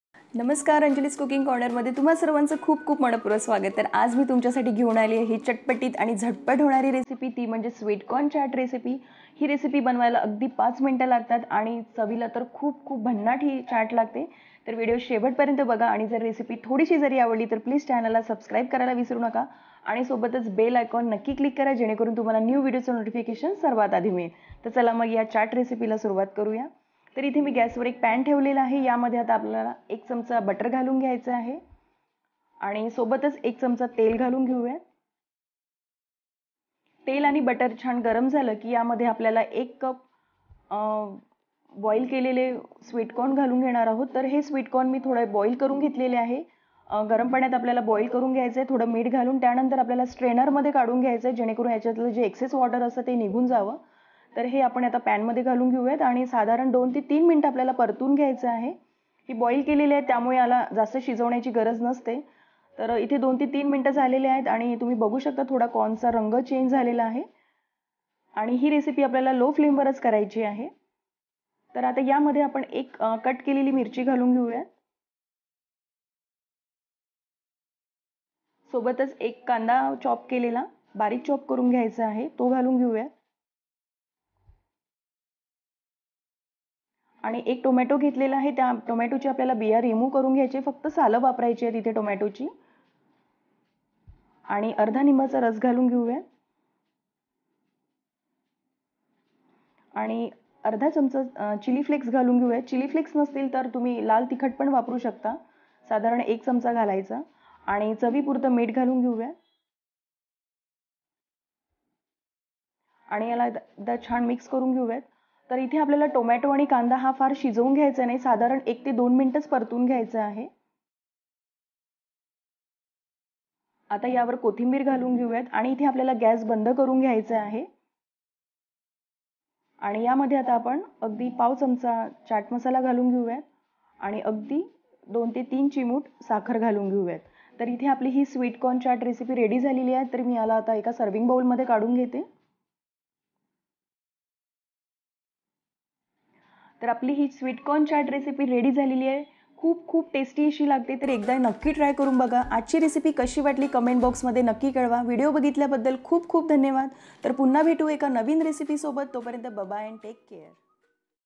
गरमा गरम चटपटीत 😋स्वीट कॉर्न चाट/Spicy Sweet Corn chat/ How To Make Butter Sweet Corn chat By Anjali
*Sweet Corn Chat Recipe In 5 Mints.*

Hello friends,
                   This is Anjali, wel come to our YouTube channel Anjali's Cooking Corner 🙏

आज आपण बघणार आहोत गरमागरम चटपटीत स्वीट कॉर्न चाट ची रेसिपी.

       Ingredients
Sweet Corn boil-1 cup
Chopped coriander
Chopped chilly -1
Chilly flecks -1 tsp.